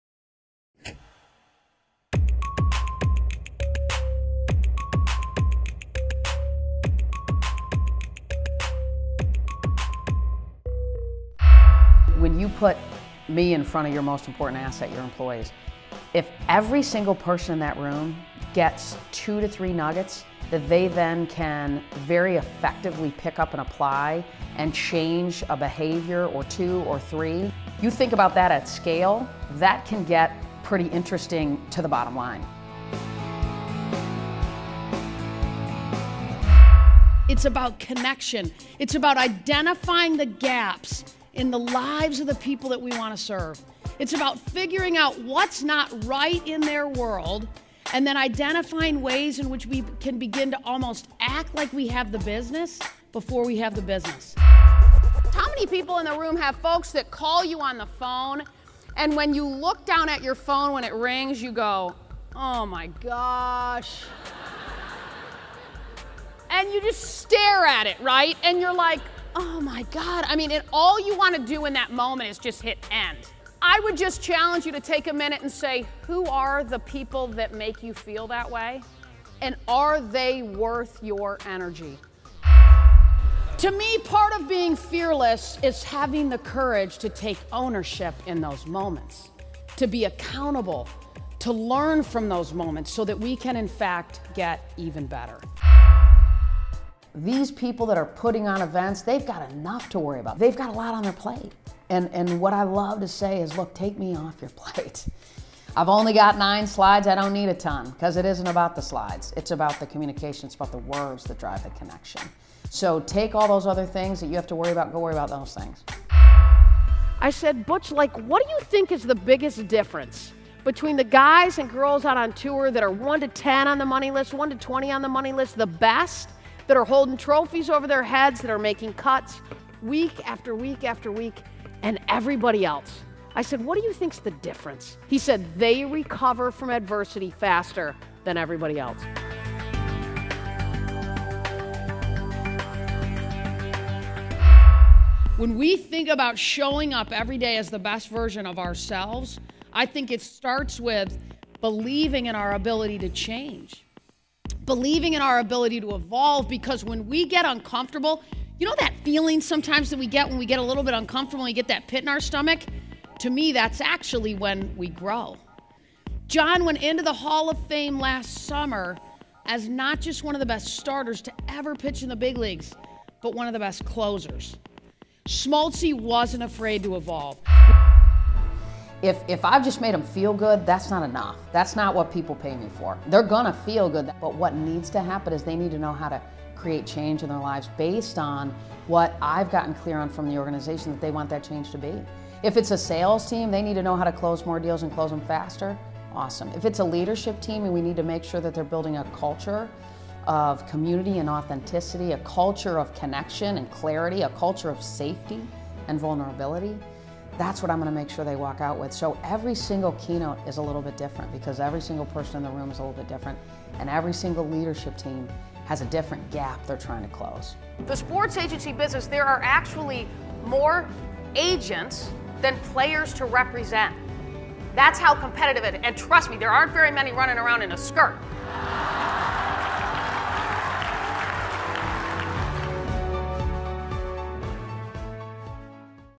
Fearless Leadership | Molly Fletcher
Molly Fletcher is a trailblazer in every sense of the word. She is a rare talent of business wisdom, relationship brilliance, and unwavering optimism. As the former president of client representation for sports and entertainment agency CSE, she was hailed as the “female Jerry Maguire” by CNN, as she recruited and represented hundreds of sport’s biggest names, including Hall of Fame pitcher John Smoltz, PGA TOUR golfer Matt Kuchar, broadcaster Erin Andrews, and basketball championship coaches Tom Izzo and Doc Rivers. Today, Molly shares her unconventional and unique techniques that made her one of the first female sports agents in the high stakes, big ego world of professional sports and now a successful entrepreneur.